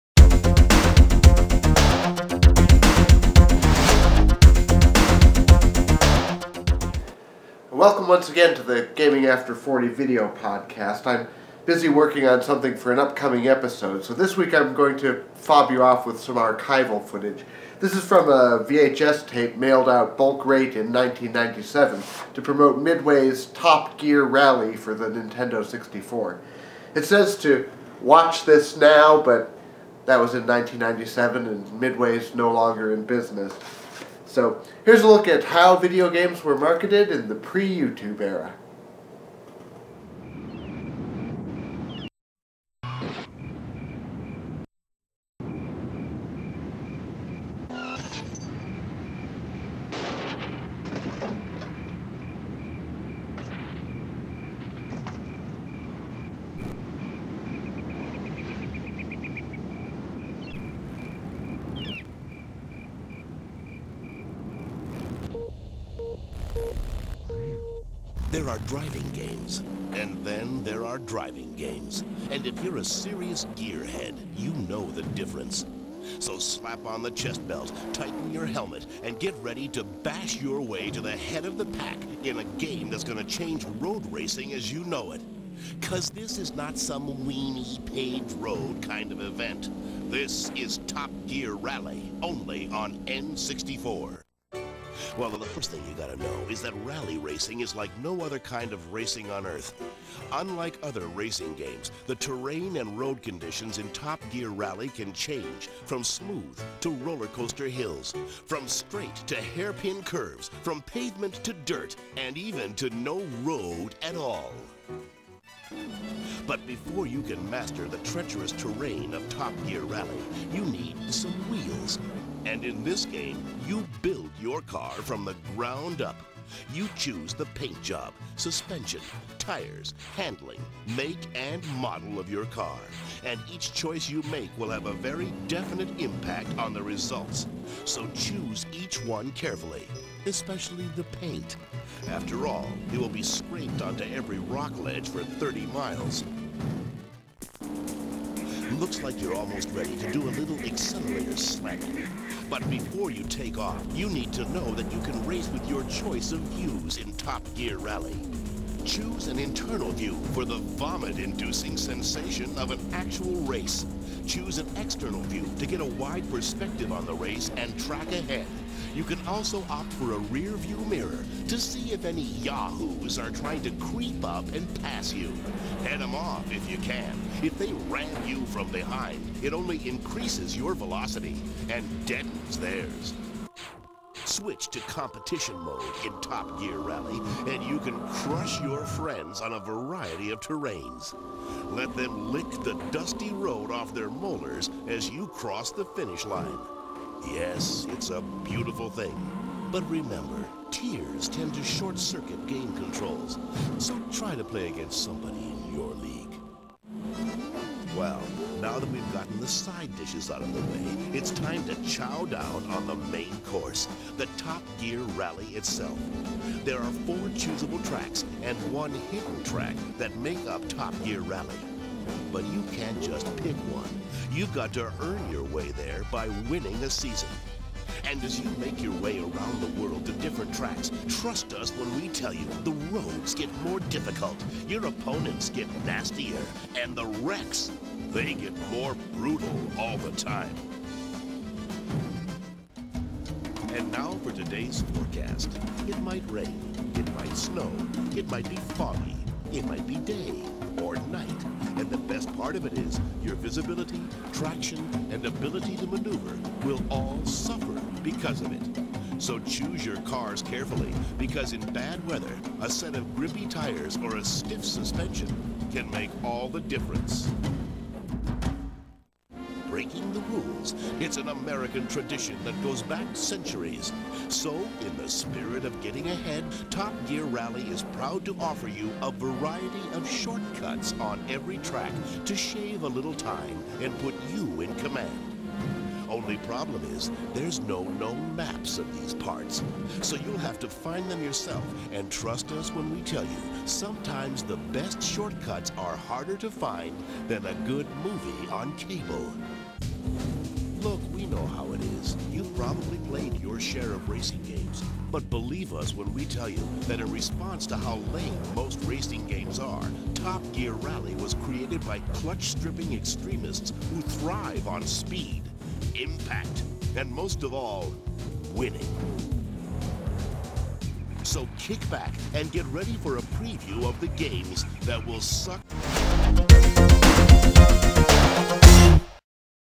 Gaming After 40 - Selling Top Gear Rally
A look at Midway's direct marketing circa 1997.